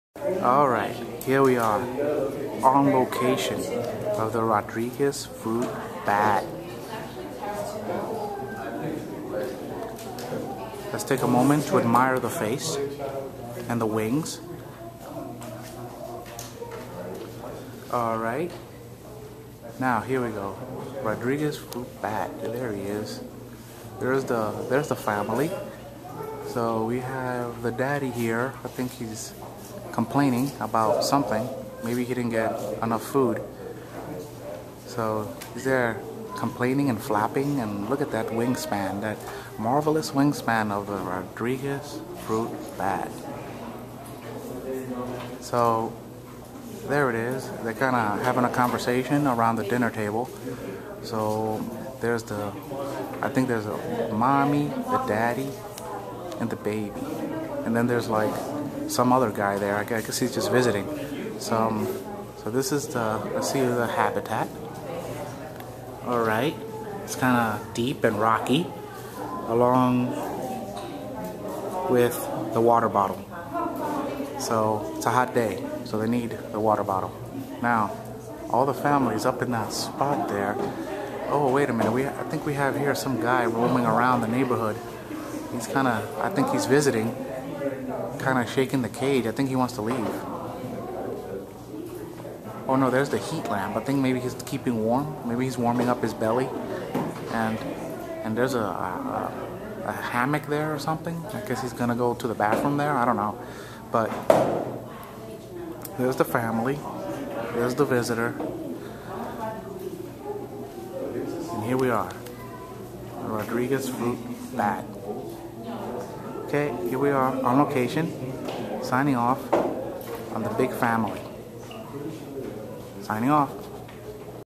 On Location: The Rodrigues Flying Fox
The Rodrigues Flying Fox or Rodrigues Fruit Bat (Pteropus rodricensis) is a species of megabat in the family Pteropodidae, the flying foxes or fruit bats. It is endemic to Rodrigues, an island in the Indian Ocean belonging to Mauritius. Its natural habitat is tropical moist lowland forests. It is a sociable species which lives in large groups. It can reach 350 grams in weight and has a wingspan of 90 centimetres. It is threatened by habitat loss through storm damage and human intervention and also by local hunting for food. Formerly the daytime roosts or 'camps' of this flying fox contained more than 500 individuals. The species currently numbers just a few hundred in the wild and is classified as critically endangered by the IUCN. The Durrell Wildlife Conservation Trust has undertaken a successful captive breeding program and there are now colonies in several zoos. Likewise, the Organization for Bat Conservation is funding an international conservation project.
At night the bats forage in dry woodland for fruit of various trees such as tamarinds, rose-apples, mangoes, palms and figs. Like many other fruit bats they squeeze out the juices and soft pulp, rarely swallowing the harder parts. Observations in captivity show that each dominant male gathers a harem of up to ten females, with which he roosts and mates. Subordinate and immature males tend to roost in another part of the camp.